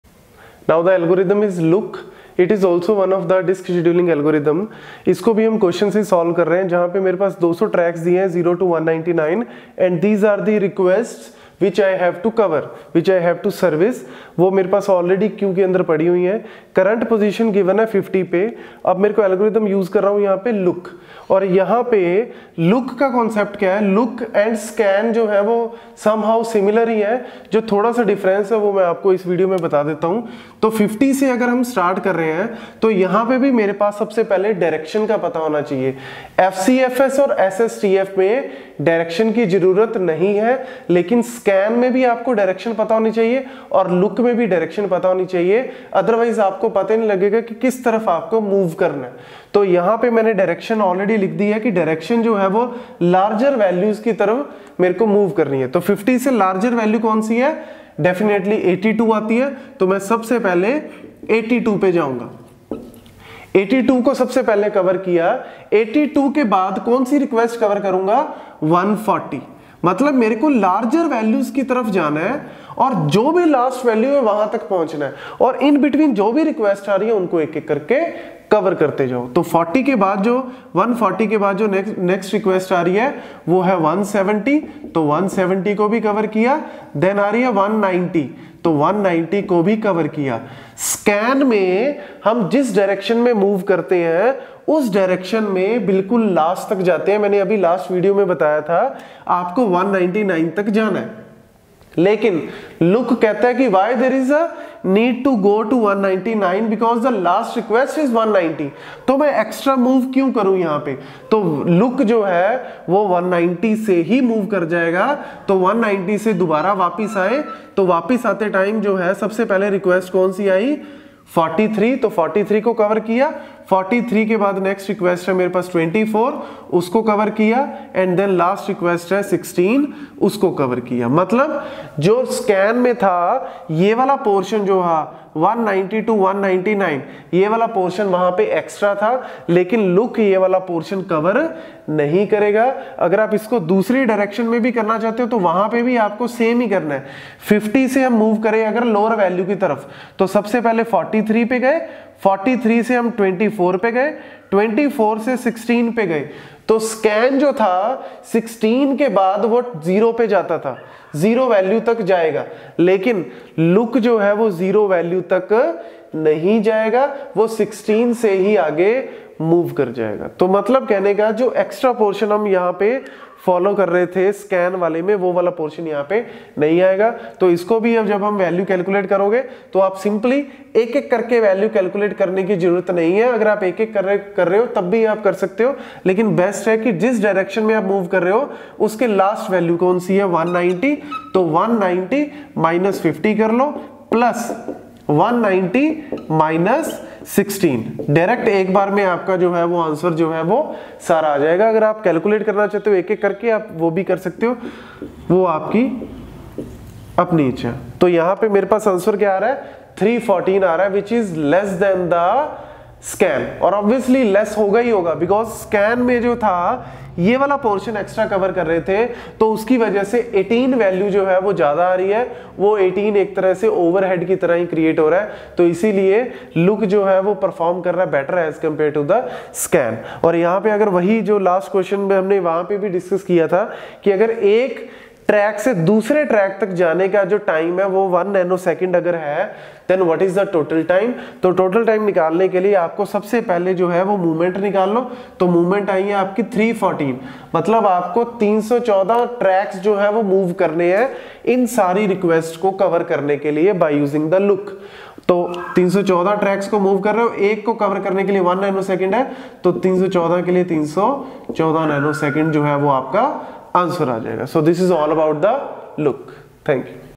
L-6.7: LOOK Algorithm in Disk scheduling with Example | Operating System
The LOOK algorithm is a disk scheduling algorithm that serves requests generated by the memory management unit. It is similar to the SCAN algorithm, as it evaluates the pending requests on both sweep directions of the disk head. In this video, Varun sir has explained it in detail.